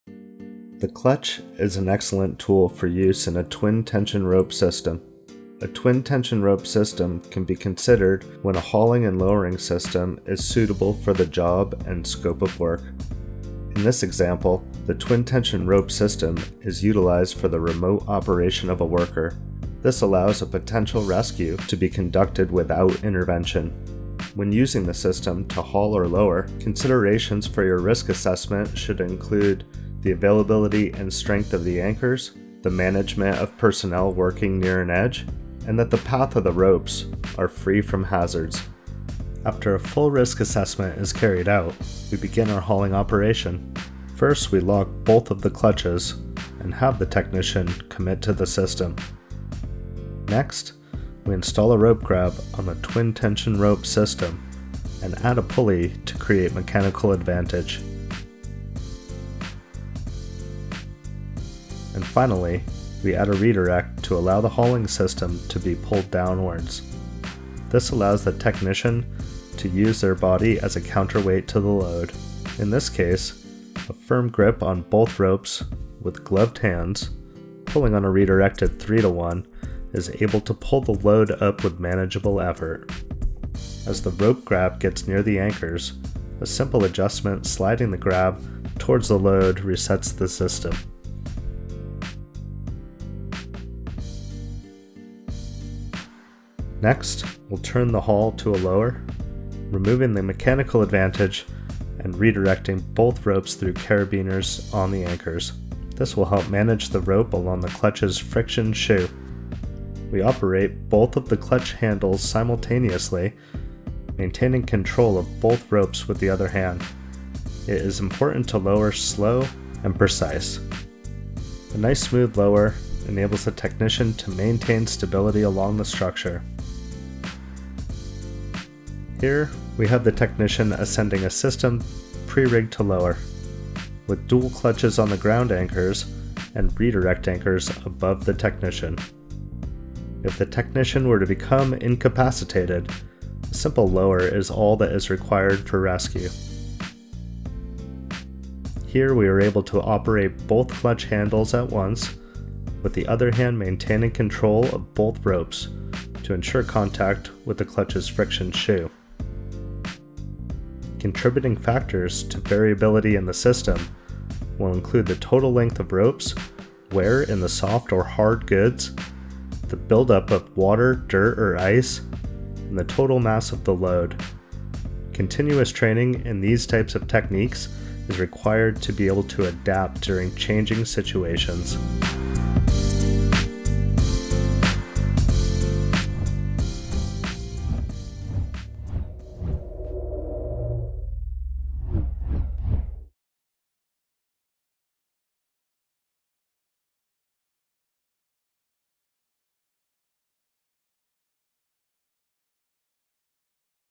Rig for Rescue Using the CLUTCH on Inverted Anchors with a TTRS - CMC Collab by Matthew Waskiewicz
00:30 Considerations for Risk Assessment
01:37 Converting the Haul to a Lower
02:08 TTRS System Pre-Rigged for Lower
02:46 Contributing Factors to Variability in the System

Matthew Waskiewicz, a SPRAT and IRATA instructor explains how the CMC CLUTCH by Harken Industrial excels as a highly-efficient, multi-purpose tool for rope access technicians. By rigging the CLUTCH for rescue on inverted anchors in a Twin Tension Rope System (TTRS), Matt is able to smoothly perform the remote operation of an incapacitated worker. This allows a potential rescue to be conducted without intervention.

Special thanks to:
Matthew Richard Waskiewicz
IRATA L3”i” Lead Instructor
SPRAT Lead Instructor / Evaluator
Technical Advisor